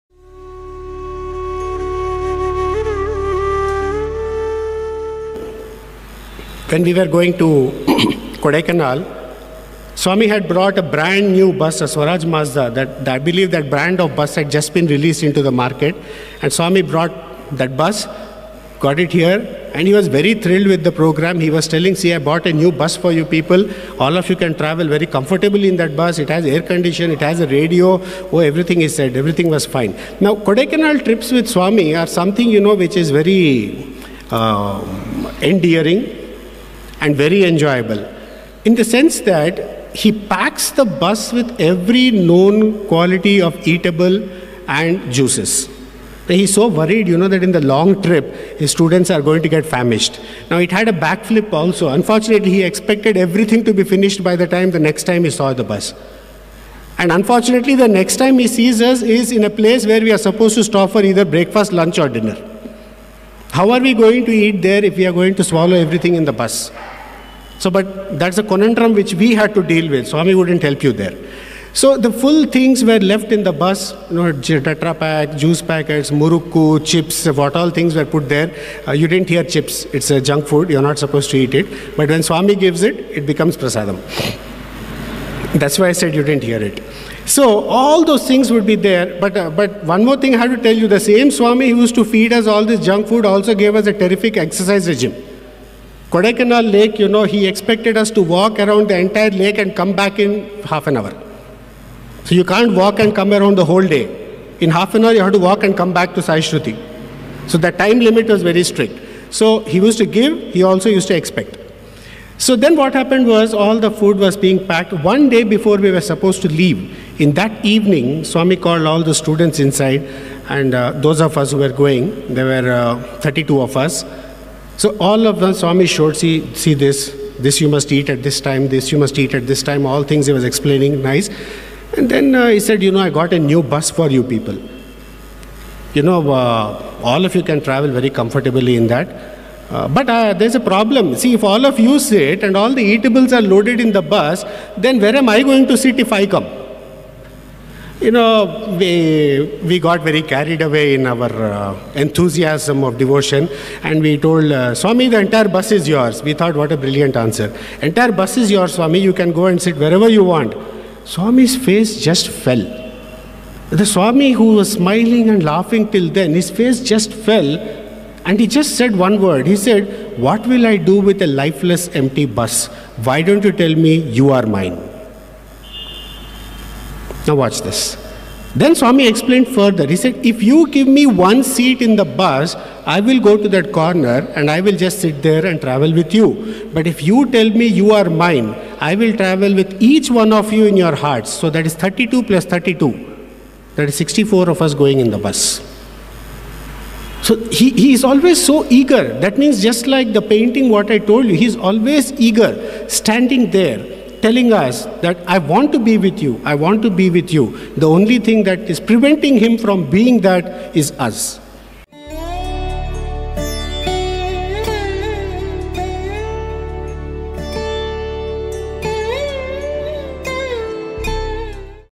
When Sri Sathya Sai Wanted to Travel by Bus with Students
When Sri Sathya Sai Wanted to Travel by Bus with Students | Talk by Dr. M. Sainath